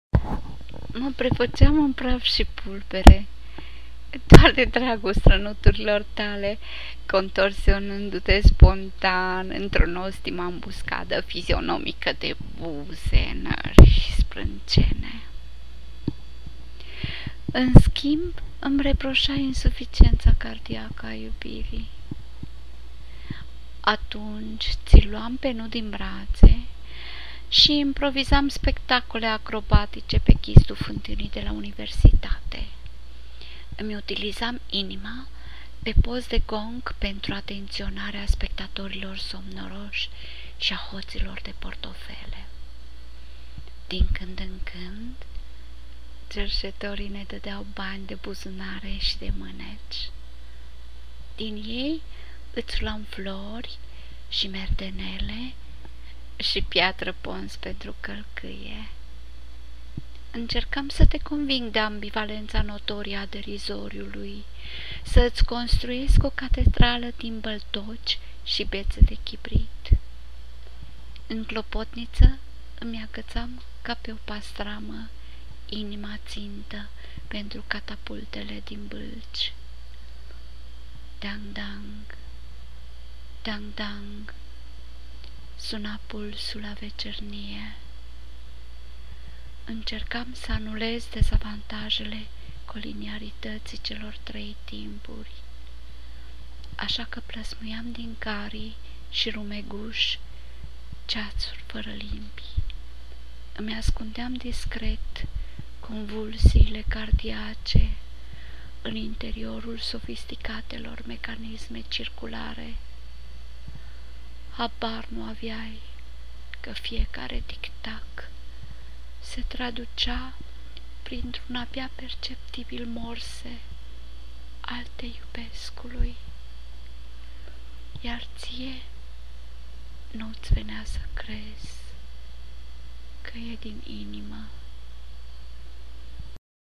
Ma prefaceam in praf si pulbere, Paul Vinicius, lectura Sonia Timariu
Ma prefaceam în praf si pulbere
doar de dragul stranuturilor tale
contorsionându-te spontan
într-o nostima ambuscada fizionomica de buze, nari, sprâncene
- în schimb îmi reprosai insuficienta cardiaca a iubirii
Atunci ti-l luam pe nu din brate
si improvizam spectacole acrobatice pe ghizdul fantanii de la universitate
Imi utilizam inima pe post de gong
pentru atentionarea spectatorilor somnorosi
si a hotilor de portofele.
Din cand in cand cersetorii ne dadeau bani de buzunare si de maneci
Din ei iti luam flori si merdenele si piatra ponce pentru calcaie.
Incercam sa te conving de ambivalenta notorie a derizoriului
sa iti construiesc o catedrala din baltoci si bete de chibrit
In clopotinita imi agatam ca pe o pastrama inima
tinta pentru catapultele din balci
Dang-Dang, Dang Dang
Suna pulsul a vecernie.
Incercam sa anulez  dezvantajele coliniaritatii celor trei  timpuri
asa ca plasmuiam din carii si rumegus ceasuri fara limbi
Imi ascundeam discret convulsiile cardiace in interiorul sofisticatelor mecanisme circulare.
Habar nu aveai ca fiecare tic-tac se traducea
printr-un abia perceptibil morse al te iubescului.
Iar tie nu-ti venea sa crezi ca e din inima

autor Paul Vinicius